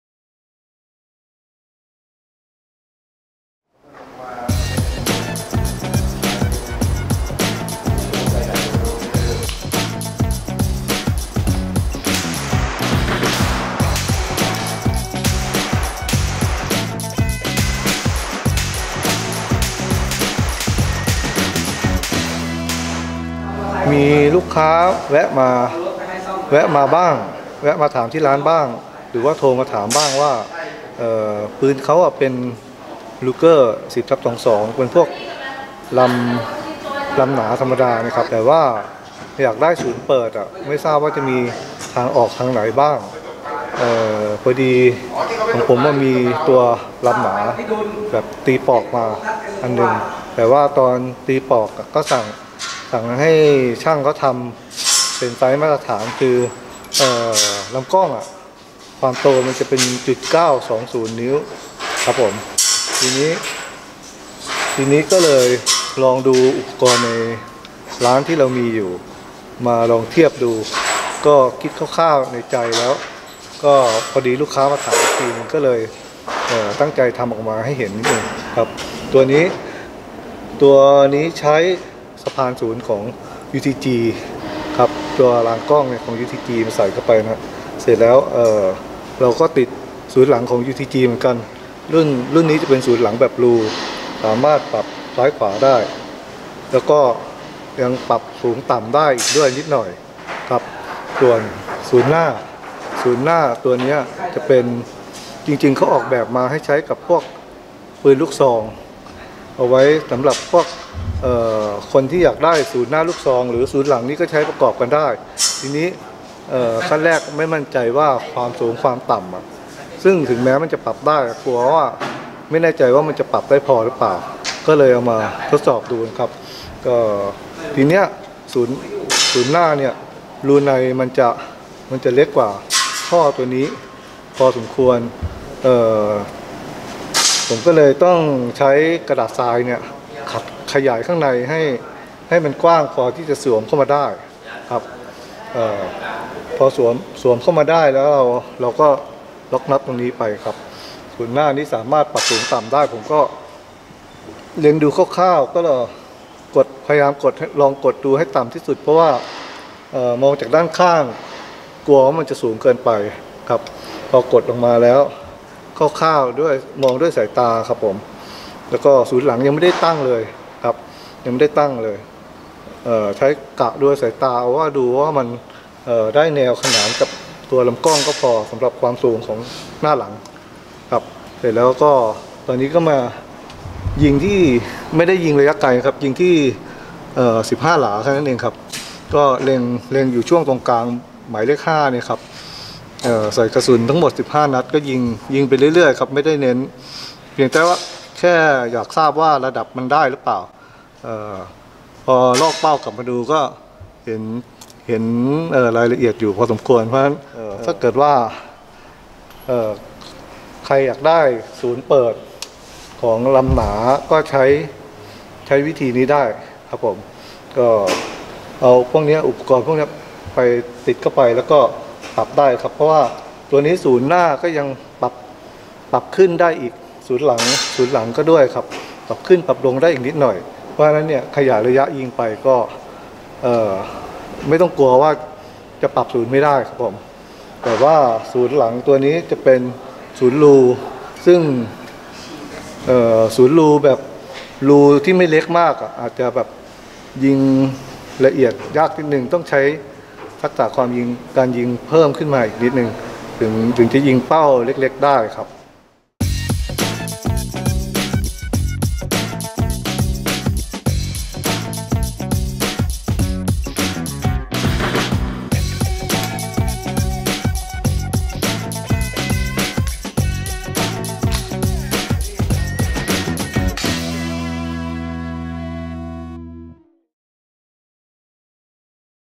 ติดศูนย์เปิดให้ Ruger 10/22 ลำหนา
ครูพระรามทดลองเอาศูนย์เปิด หน้า/หลังของ UTG มาติดกับปืน Ruger 10/22 ลำ Bull Barrel สำหรับท่านที่อยากได้ศูนย์เปิดครับ
รายละเอียดเพิ่มเติมศูนย์เปิดติดราง Picatinny
สอบถามเพิ่มเติม line @outdoorvision ครูพระราม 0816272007